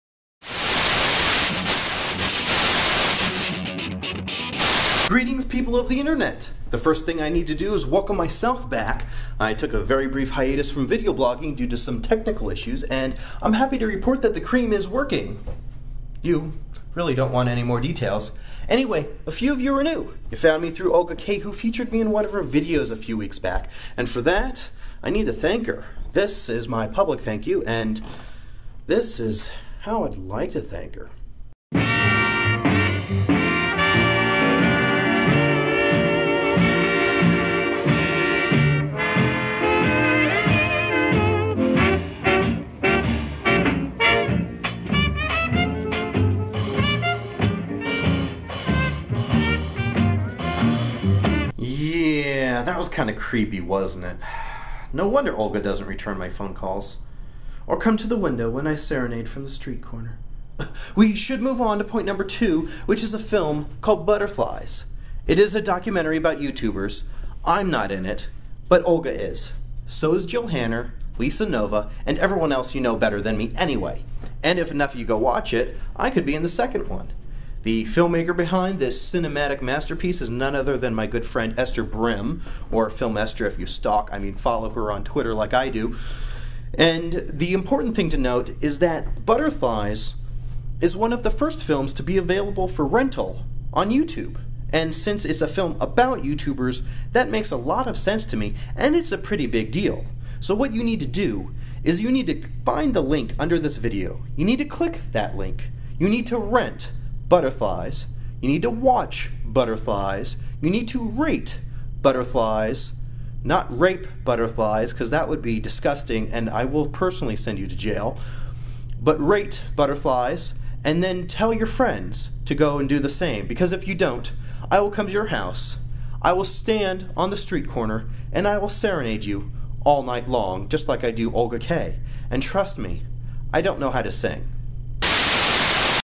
Butterflies: Rent it on YouTube!
People of the internet: I am back from my vlogging hiatus!!!

I'd like to welcome my new subscribers and thank Olga Kay for featuring me in her video a few weeks back. (A *BIG* "Moosh!" to her!). Also: my friend (and talented filmmaker), Ester Brym, directed a documentary about YouTubers titled "Butterflies", which is now available for rent right here on YouTube! Pretty fitting, huh? You should go watch it... or bad things will happen.